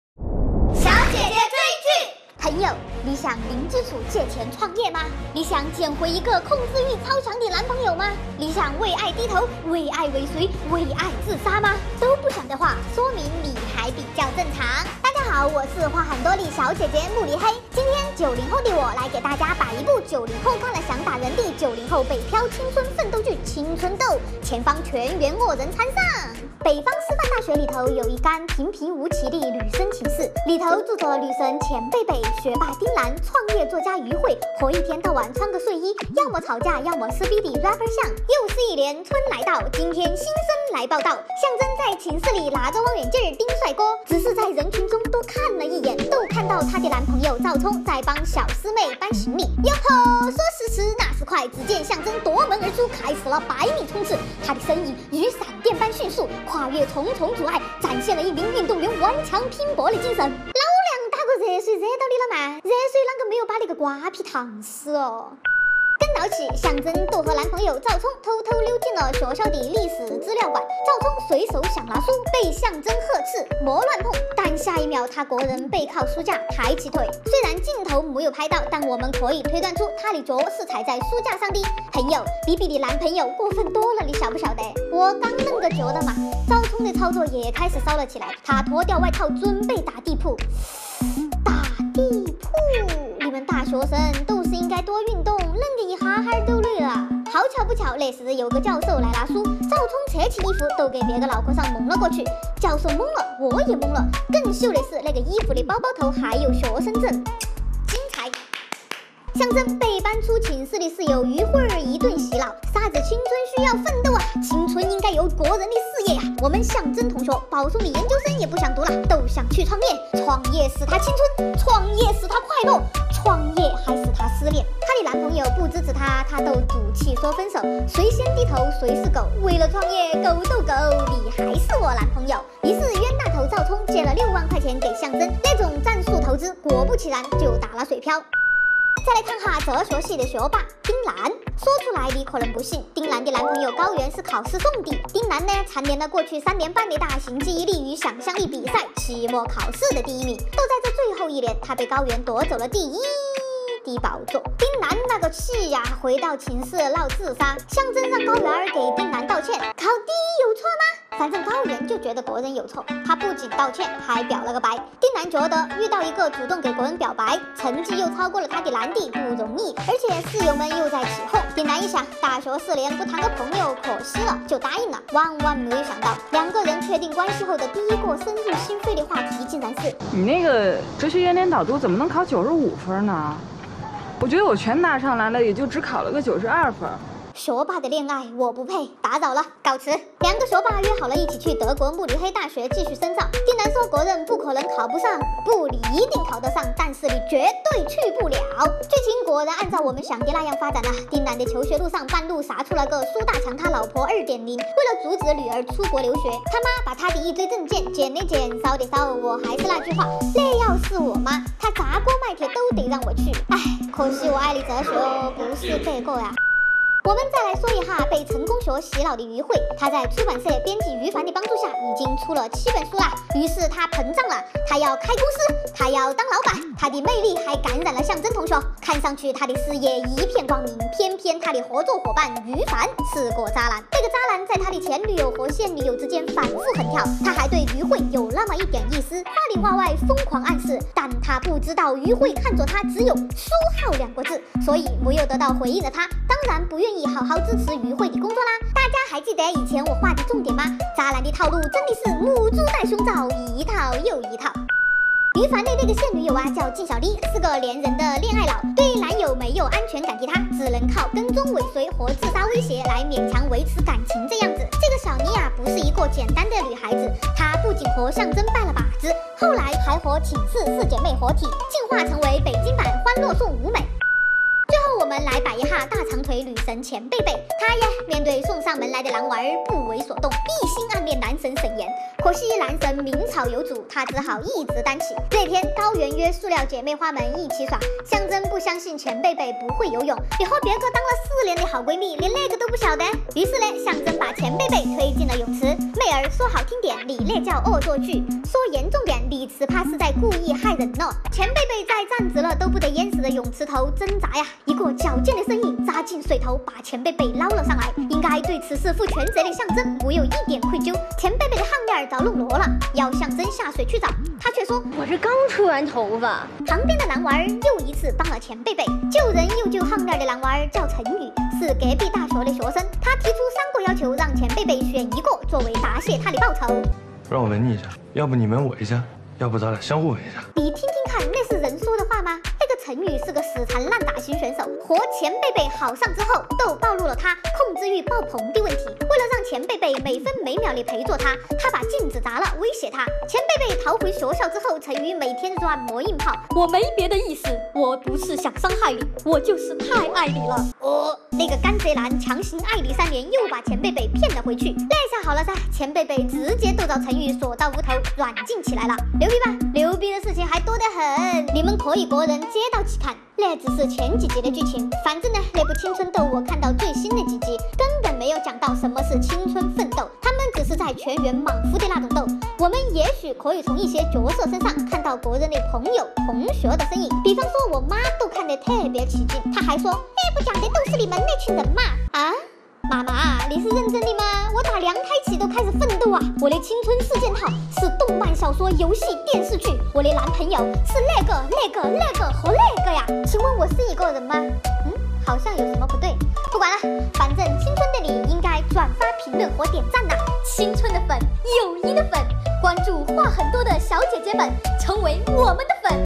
吐槽伪青春奋斗剧《青春斗》：披着励志的外衣，演着狗血的套路丨话很多的小姐姐们JustTalking
这部剧就是在违法的边缘来回试探，真的很“逗”（没有说女演员不好的意思）